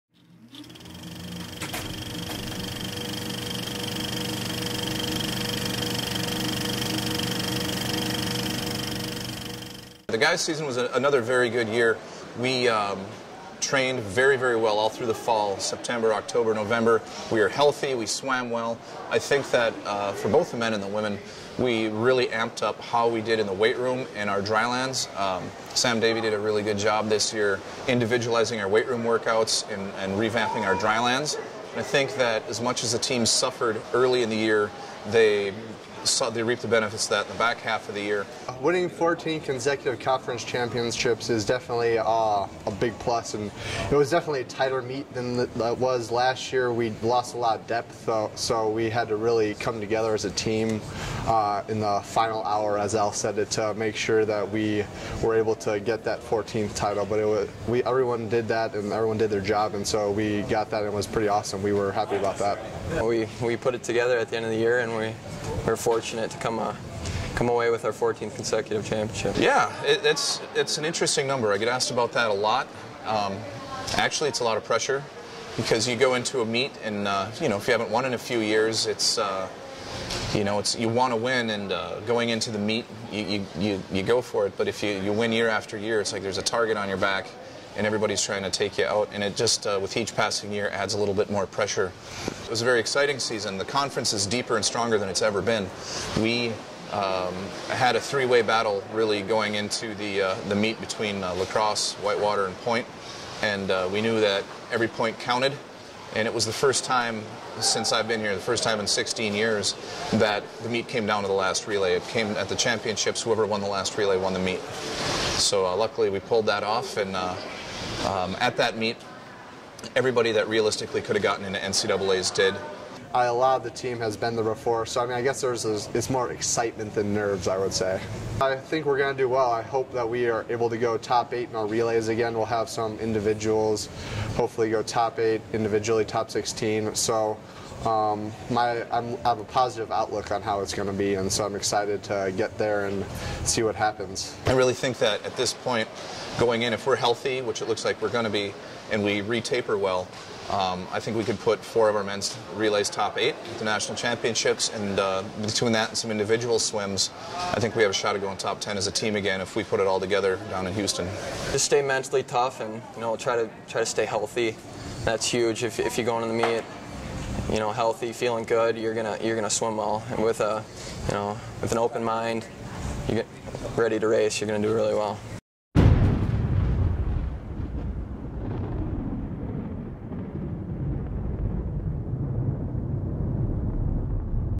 Men's Swimming and Diving - 14 Consecutive WIAC Championships
The WIAC Champions get ready for nationals